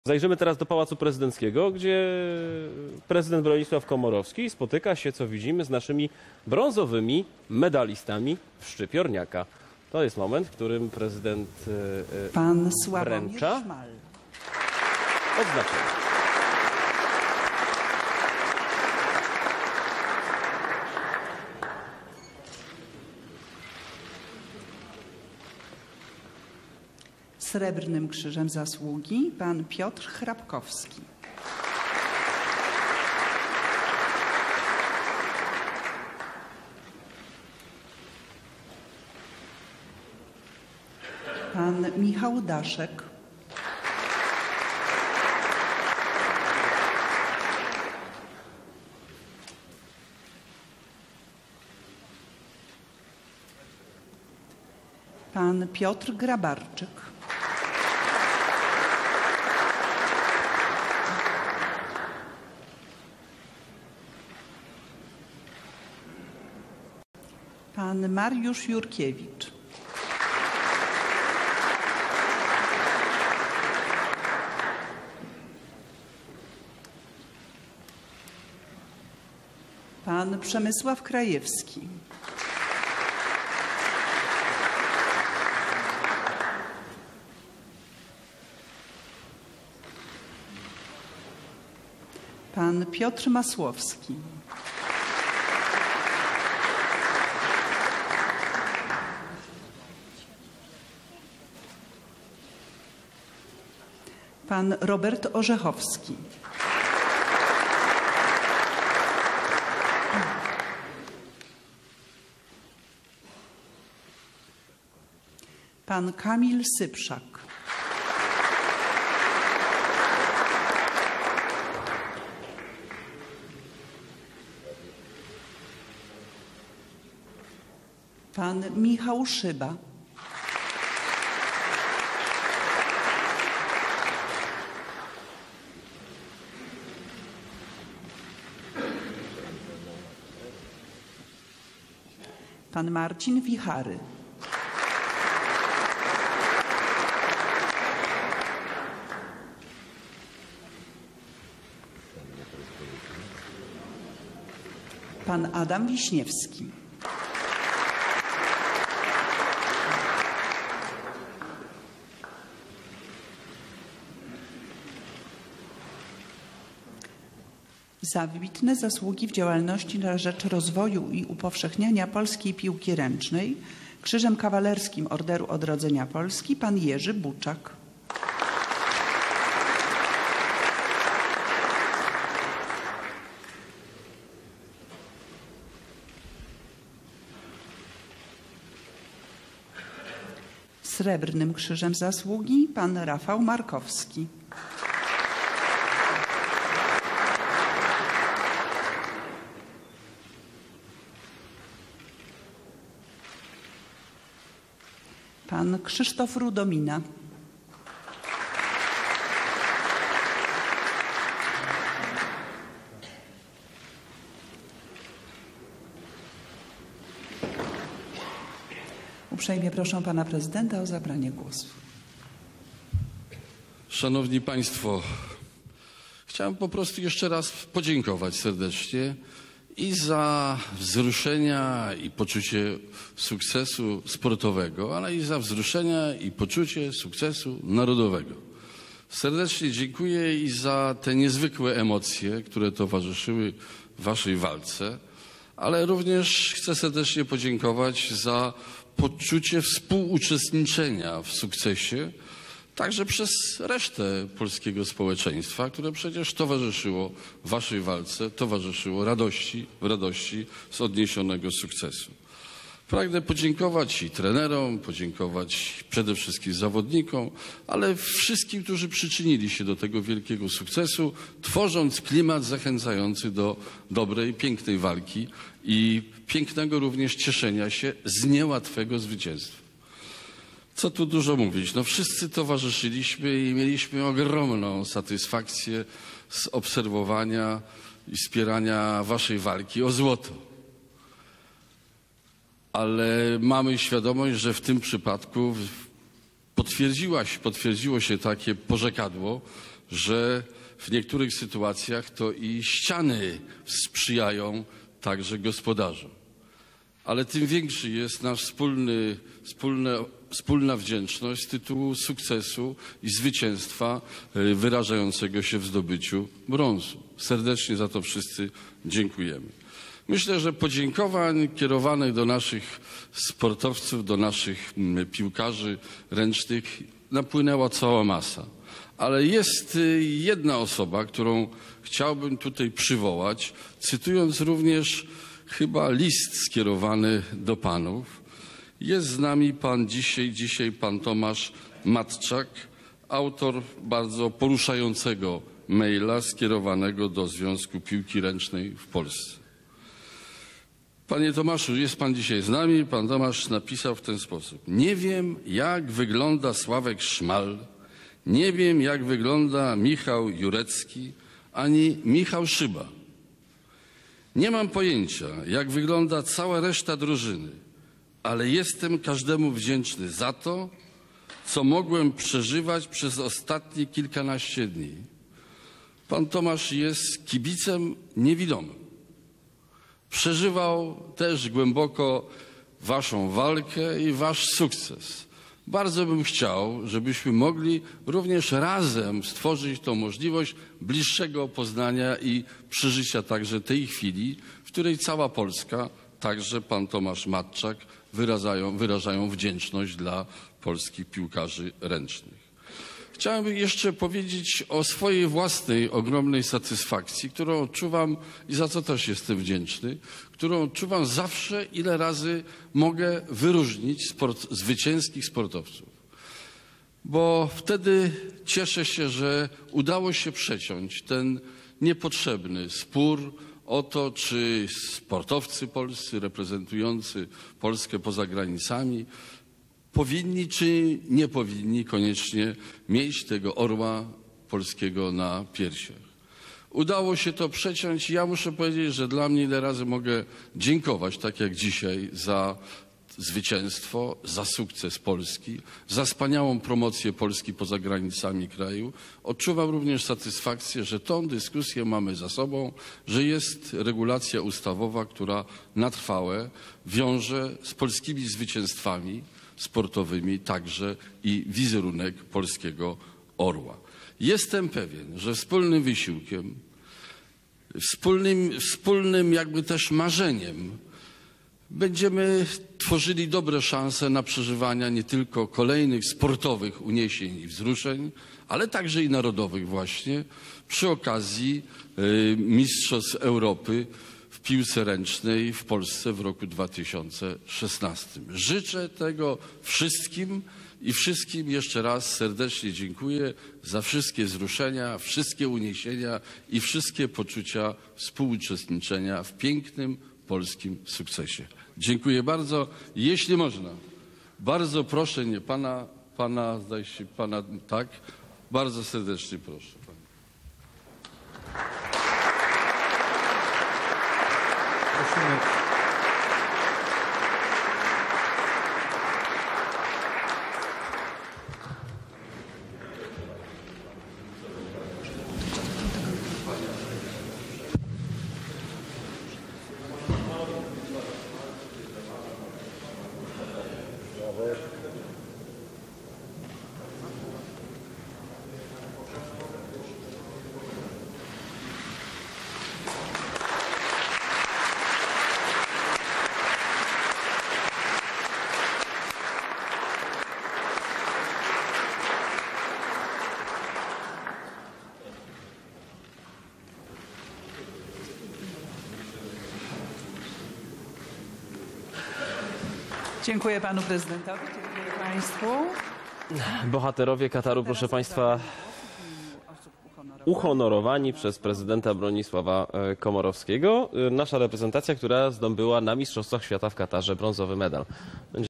Niewidomy kibic napisał list do piłkarzy ręcznych. Został wyróżniony przez prezydenta
Bronisław Komorowski odznaczył brązowych medalistów mistrzostw świata.